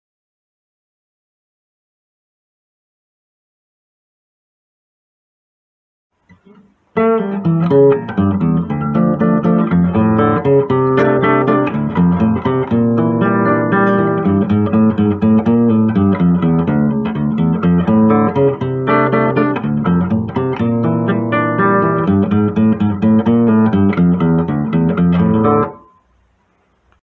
Estudando os Mestres no Violão de 7 Cordas - Rogério Caetano - Exemplo 6 Ré Menor
Estudo do livro Sete cordas técnica e estilo de Rogério Caetano